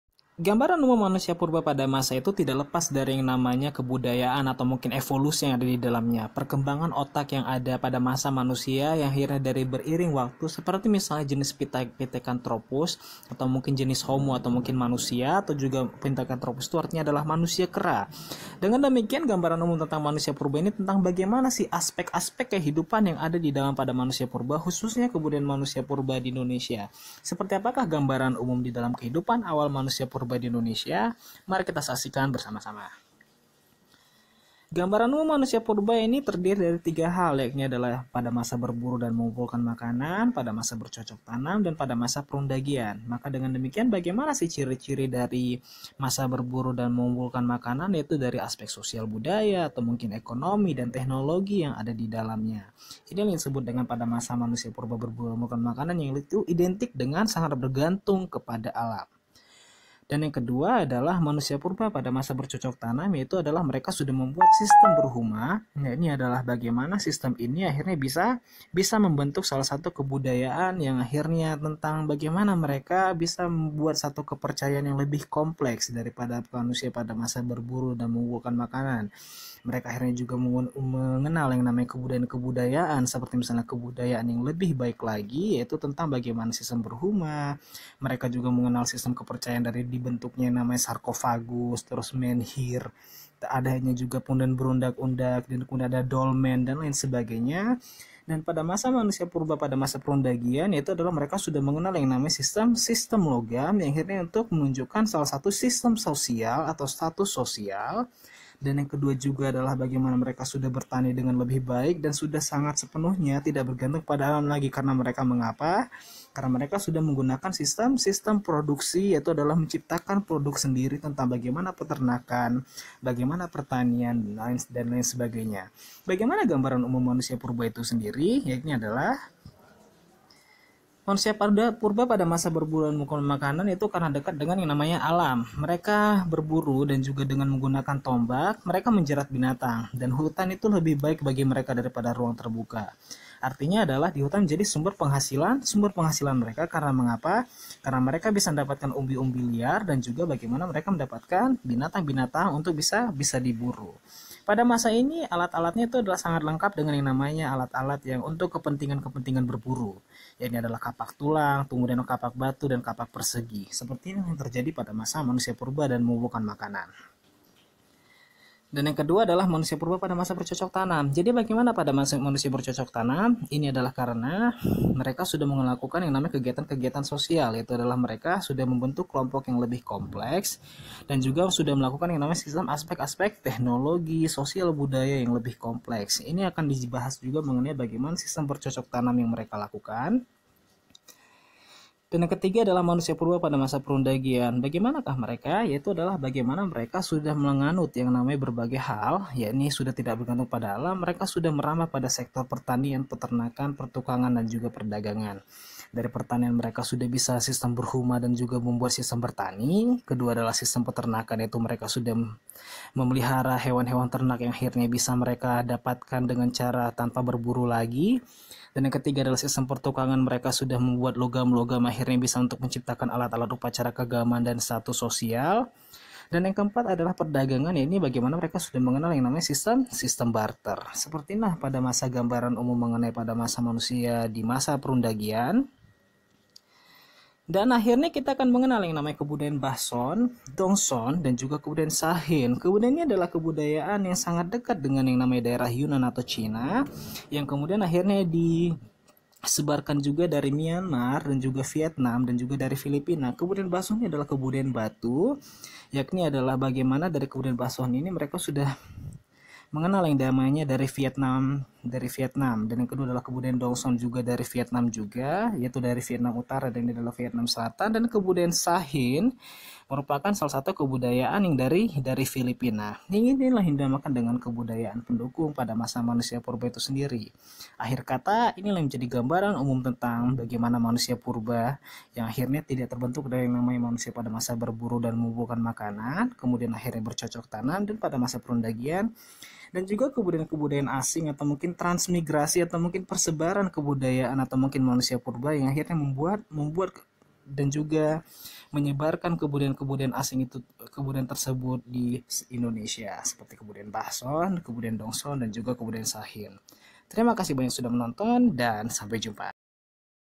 Gambaran umum manusia Purba di Indonesia (Sejarah - SBMPTN, UN, SMA)
Menjelaskan Gambaran umum kehidupan awal manusia purba di Indonesia.